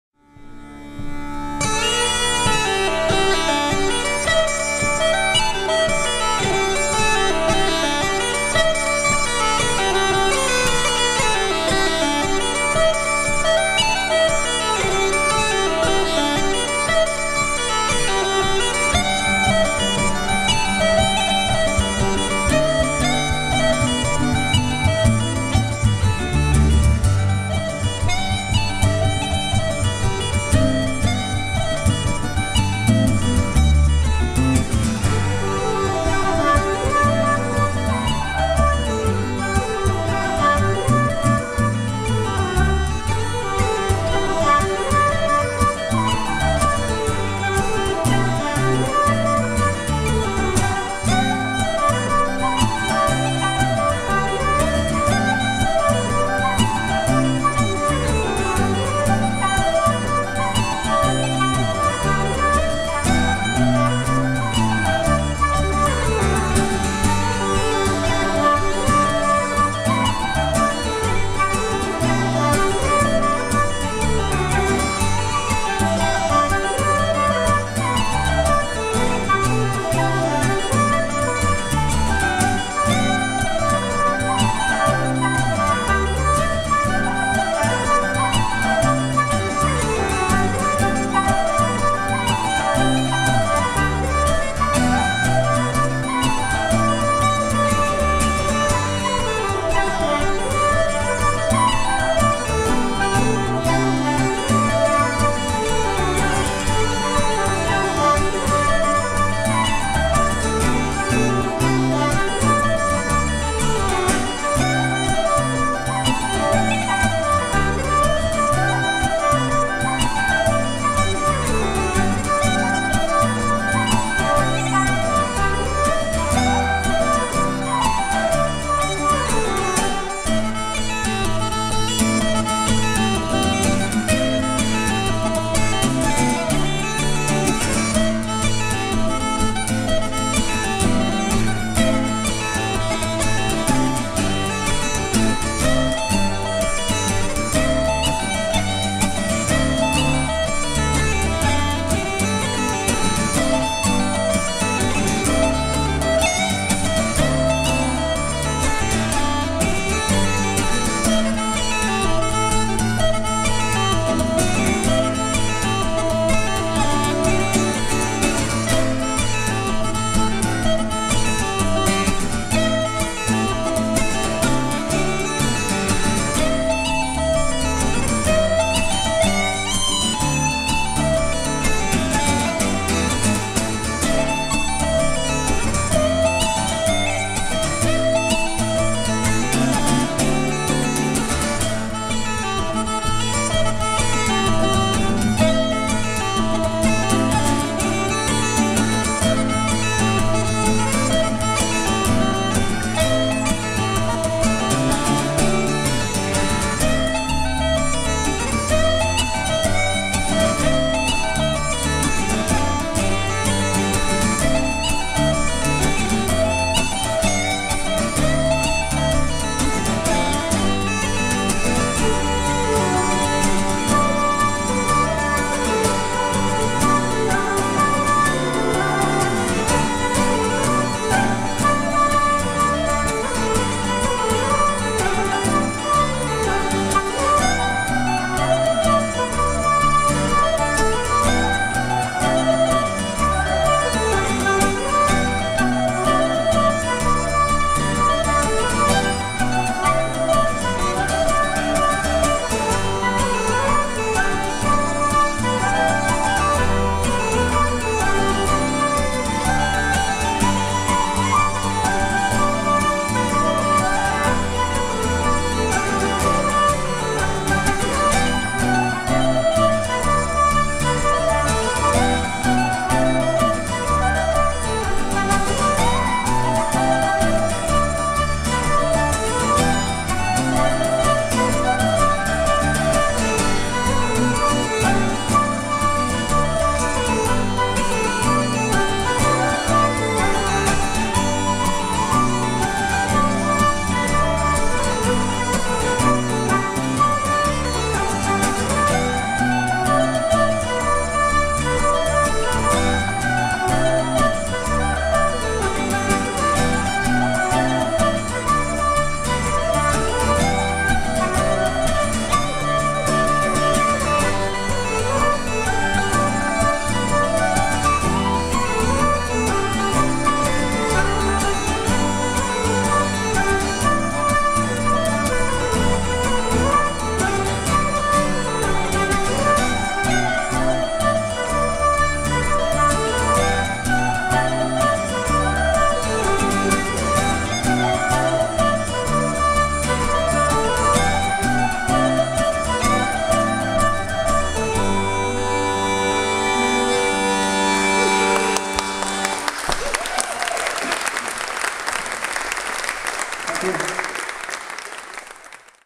15/09/11 Paddy Keenan, James Riley and Barra McAllister at Steeple Sessions 2011
Paddy Keenan playing the uilleann pipes, James Riley playing the guitar and Barra McAllister playing the flute at the Steeple Sessions on September 15, 2011 at the Unitarian Church in Dublin.